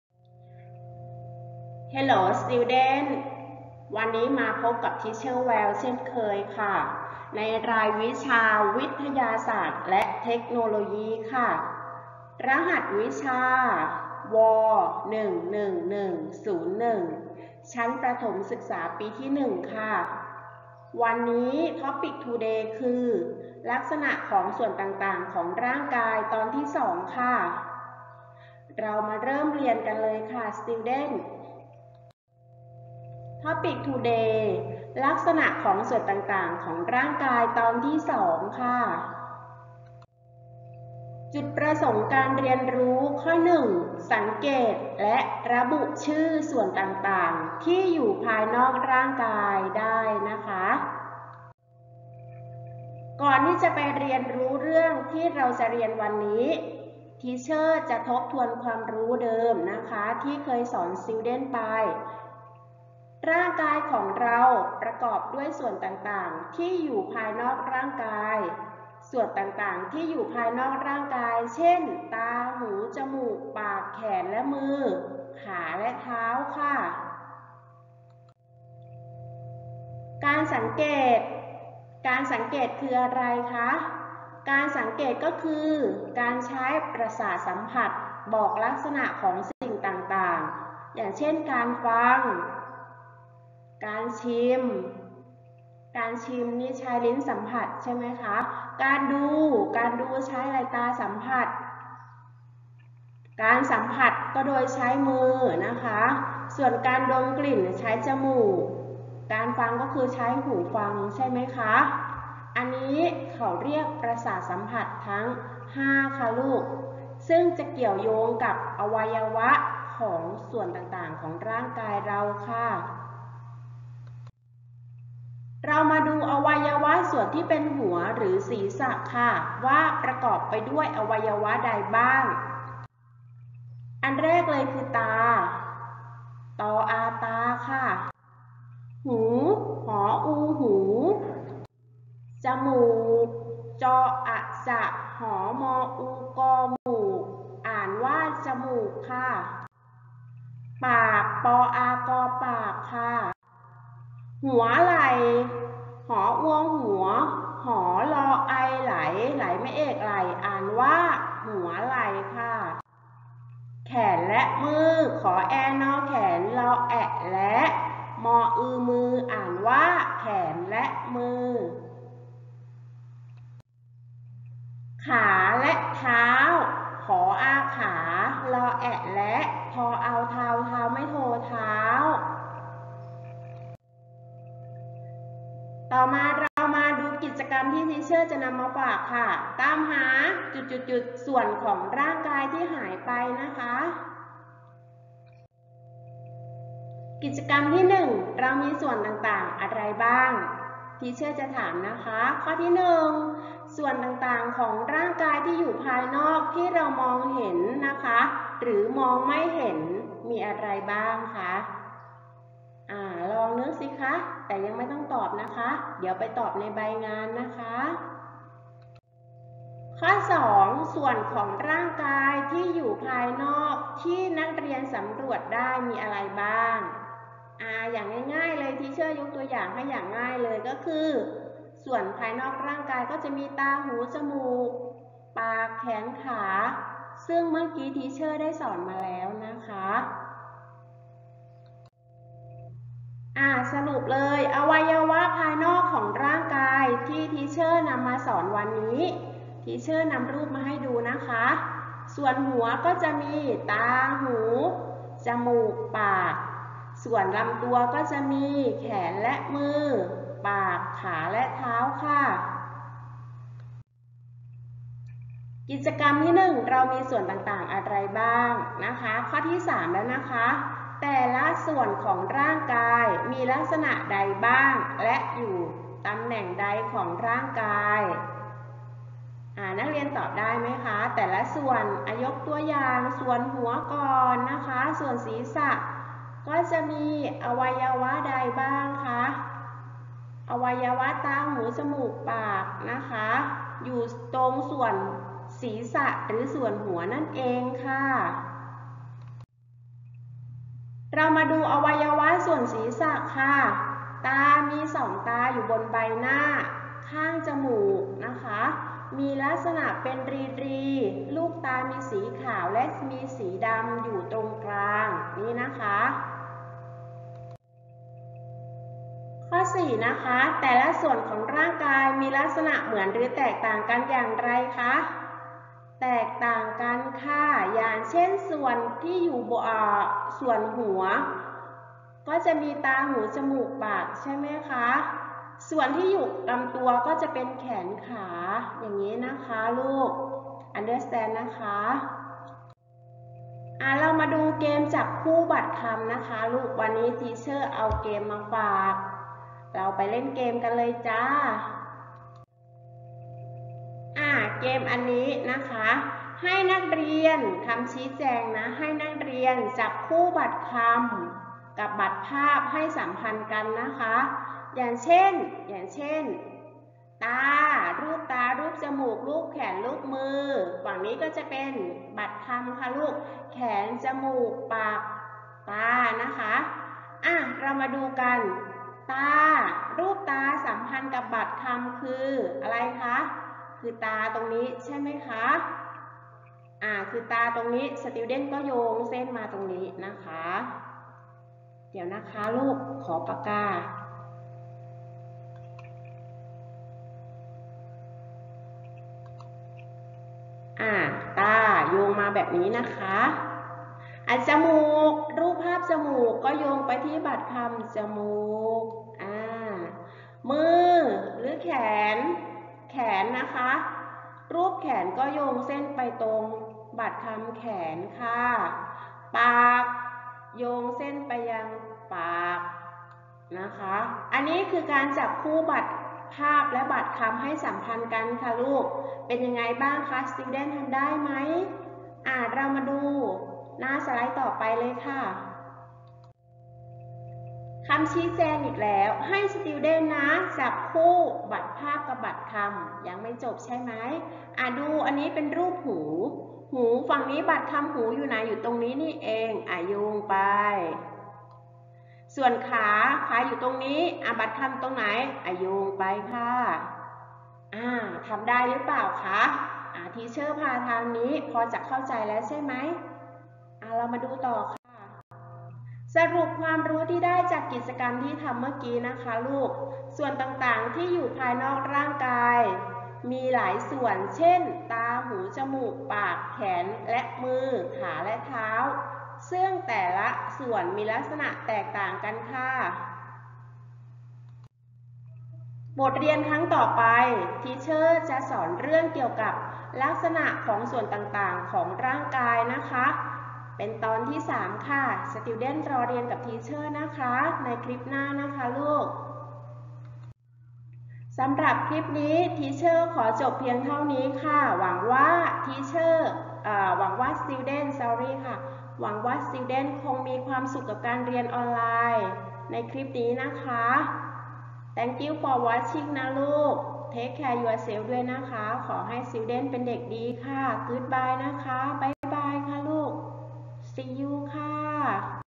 Grade 1 : Science Subject (Reg) || Week 13 : ลักษณะของส่วนต่าง ๆ ของร่างกาย (2)
รายวิชา วิทยาศาสตร์  ระดับชั้นประถมศึกษาปีที่ 1 (Reg) สัปดาห์ที่ 13
หัวข้อ : ลักษณะของส่วนต่าง ๆ ของร่างกาย (2)